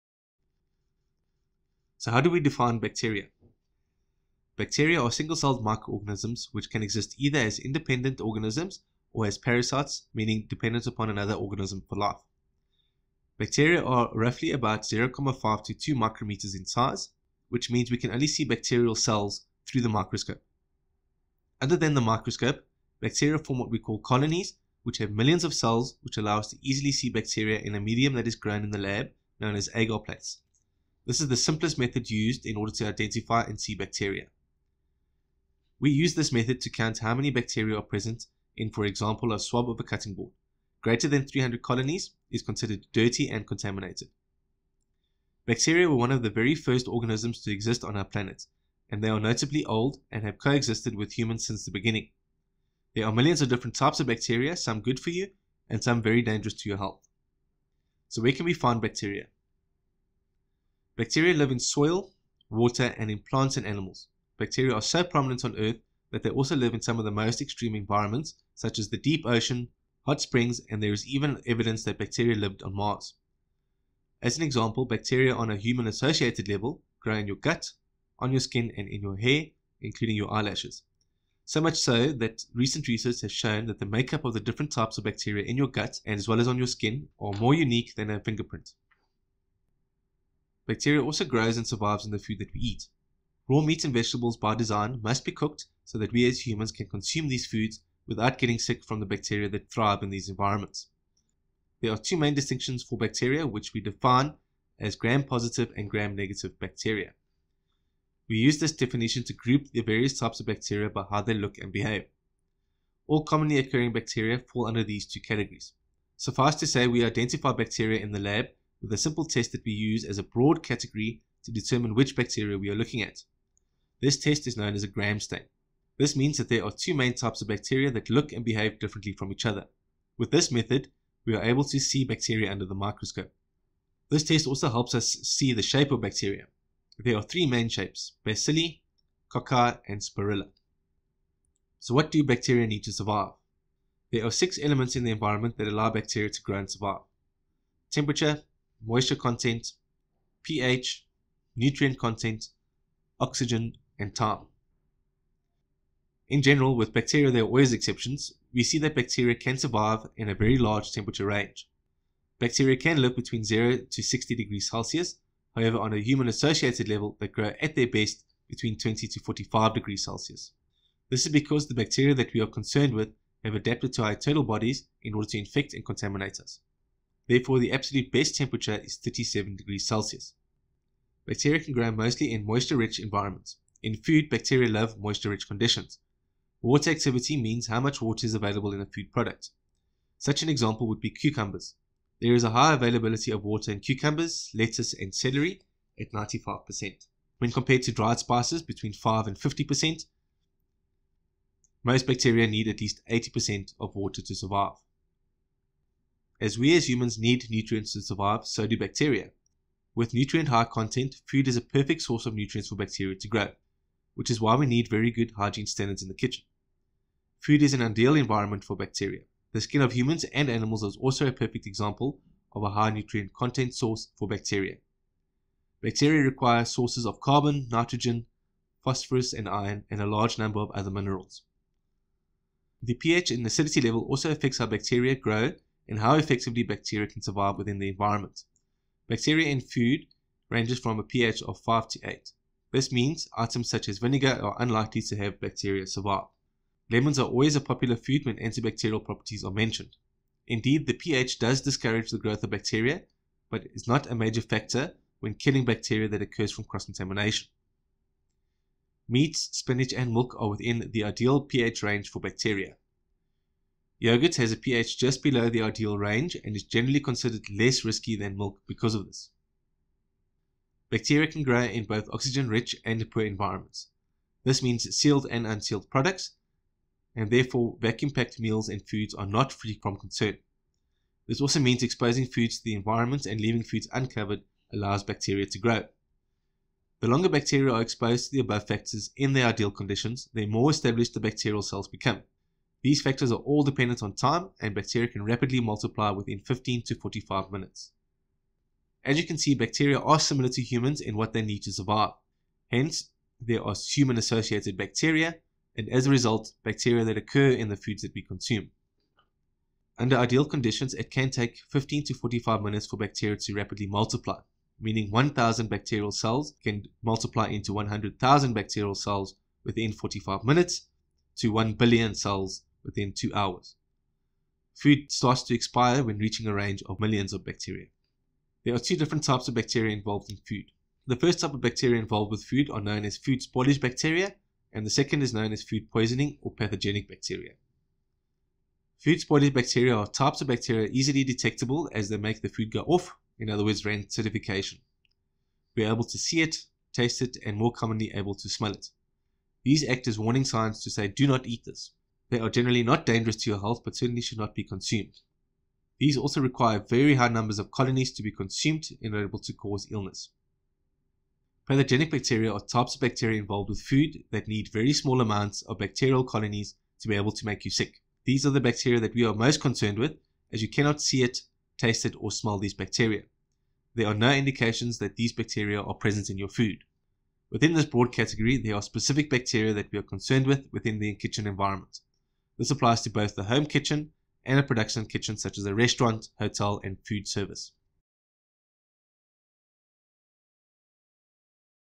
What are bacteria and what are the types of bacteria that survive in food?
In this video we discuss what bacteria are, how we define bacteria, where we can find them. We also go into detail on what bacteria needs to survive.